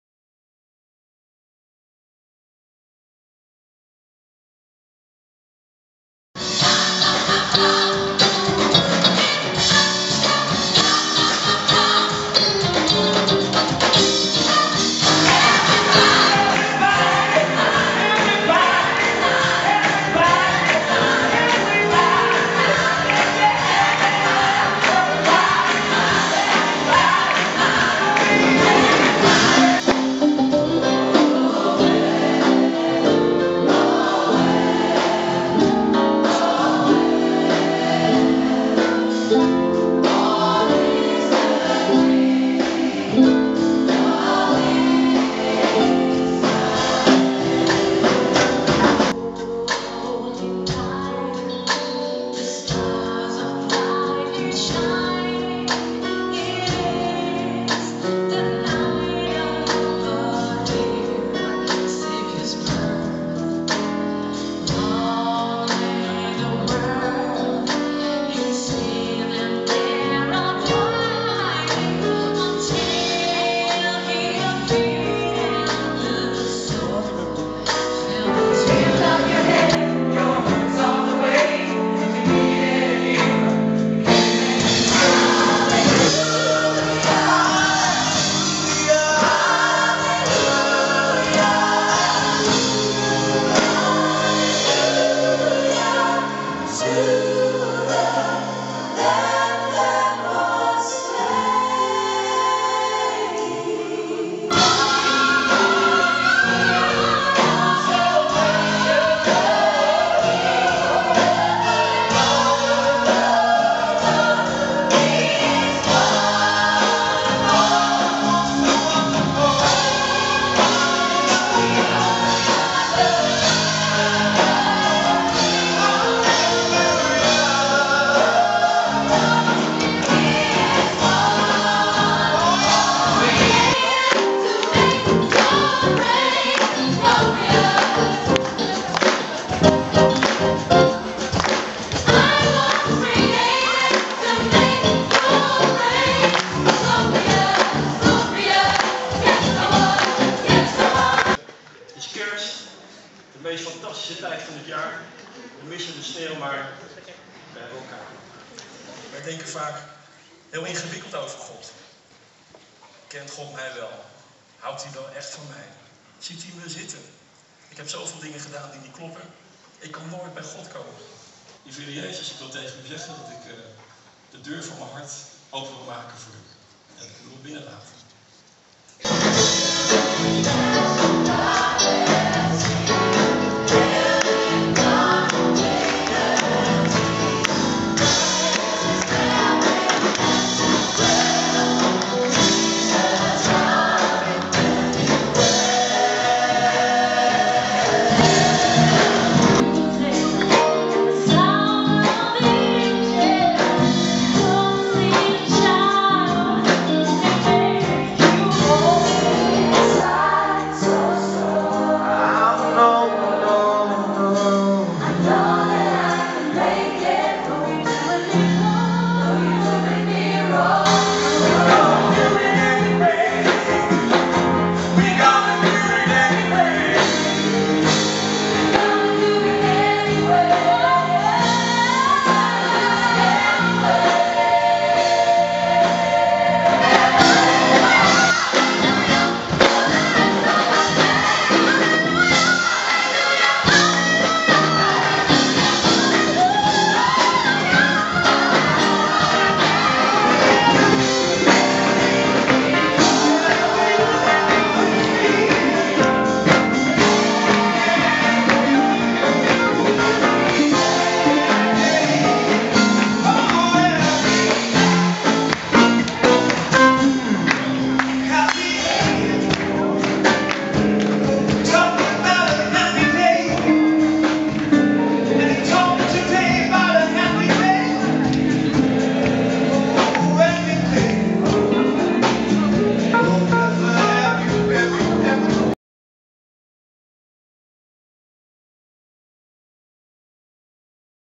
Kerstnachtdienst 2011 Immanuel
Kerstnachtdienst 2011 in Pinkstergemeente Immanuel te Haarlem.